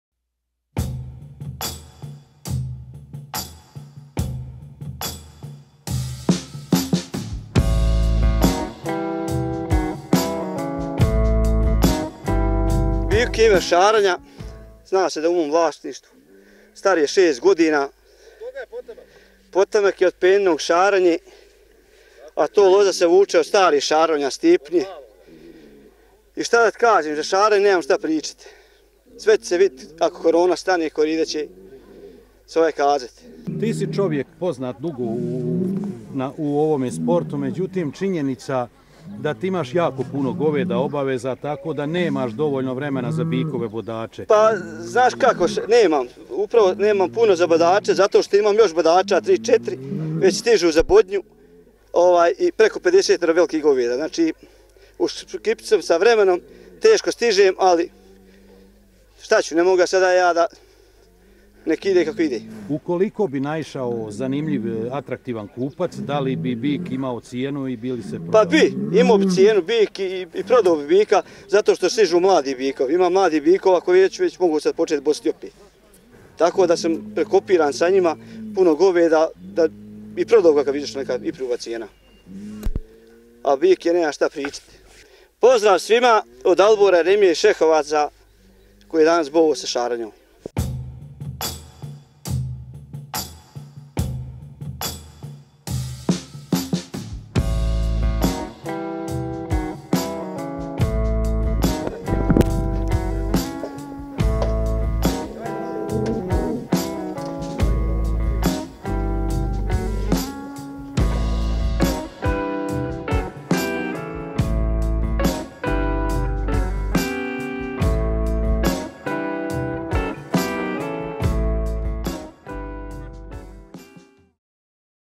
- ŠARONJA NA PRODAJU VL DALIBOR EREMIJA MRKONJIĆ GRAD 2021
Reklamn sadržaj:  Poljoprivrednik i stočar Dalibor Eremija iz Šehovaca prodaje bika Šaronju. Zainteresovani se mogu obratiti na broj: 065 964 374;